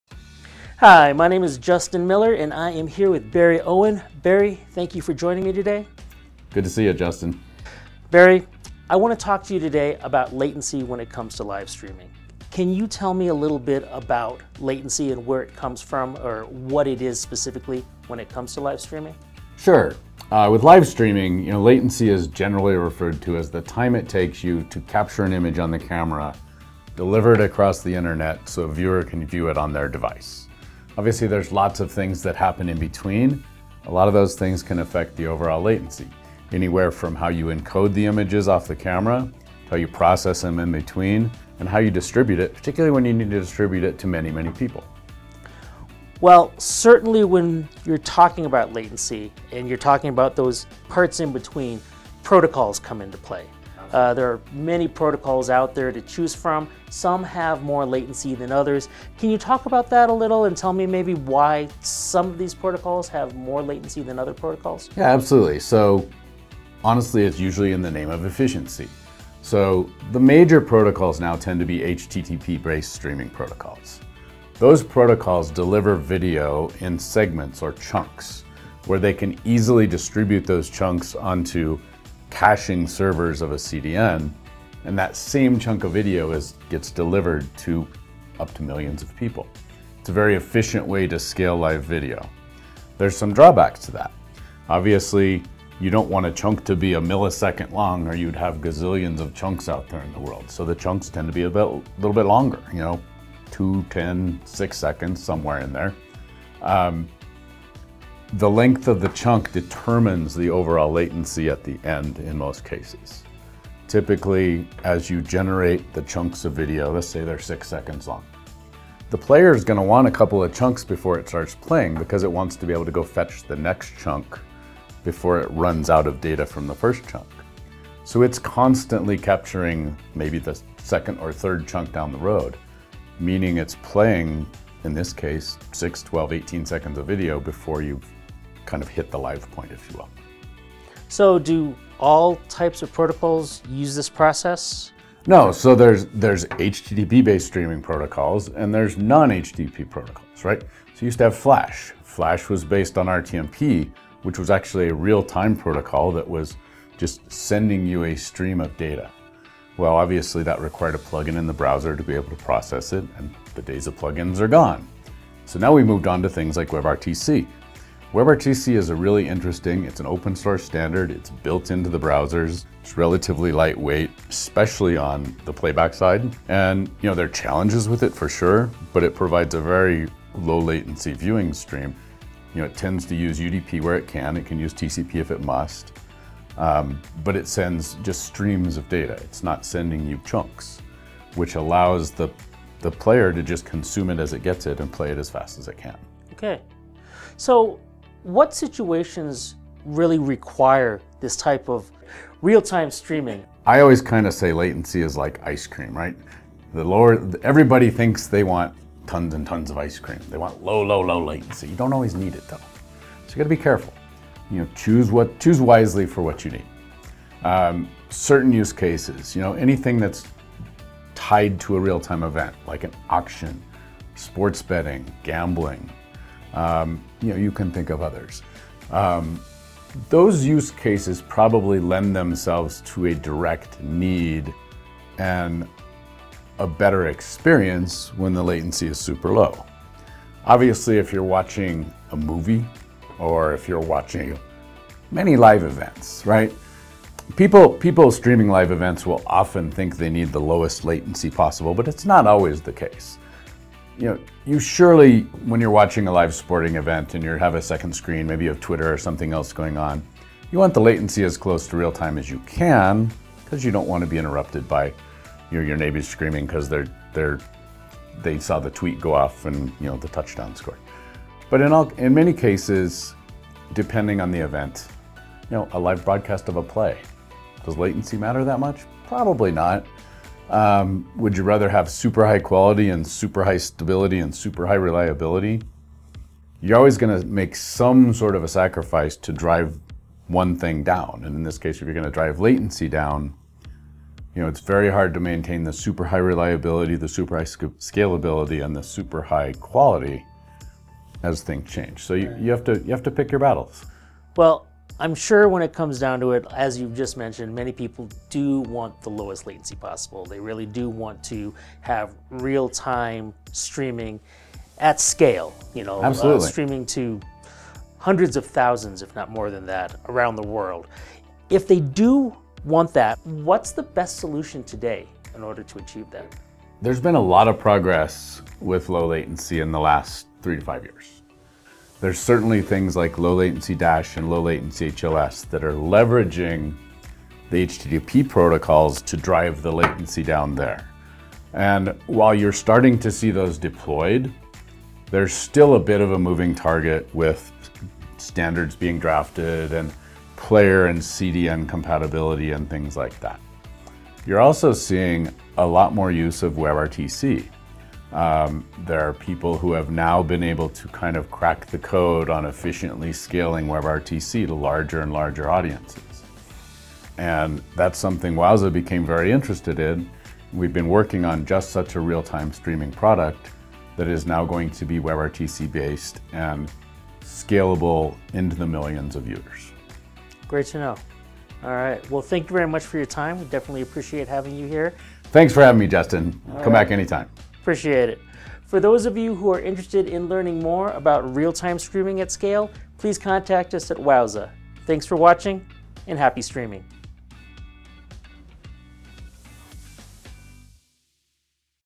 Latency in Live Streaming
Learn about what latency in live streaming is, how it occurs, and if it matters for your broadcast. for more details about low latency streaming.